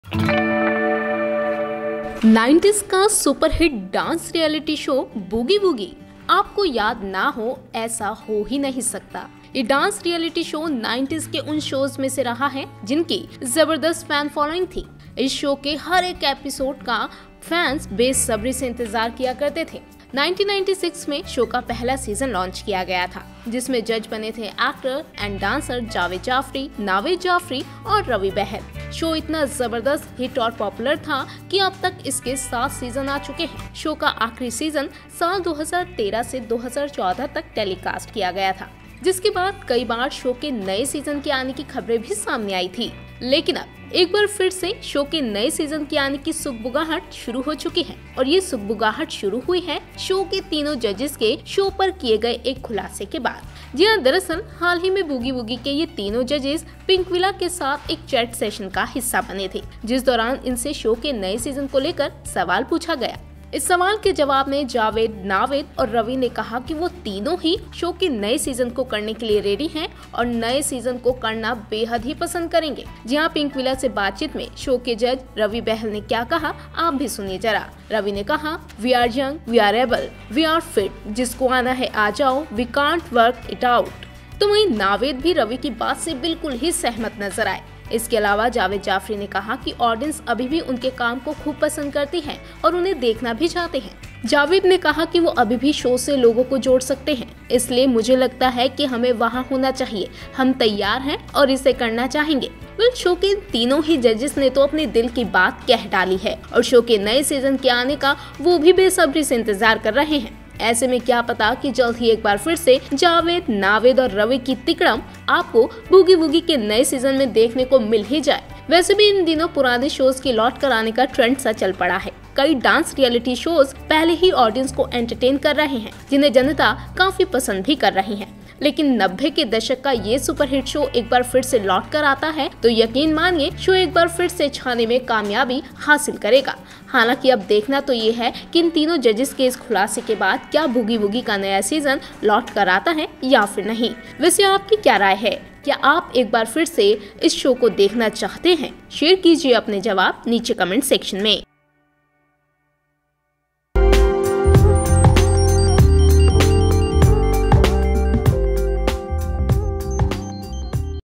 Superhit Dance Reality Show Boogie Woogie is All Set To Return?
Jaaved Jaafri-Ravi Behl and Naved Jafri to team up for Boogie Woogie’s next season? Check Out What They Said on New Season!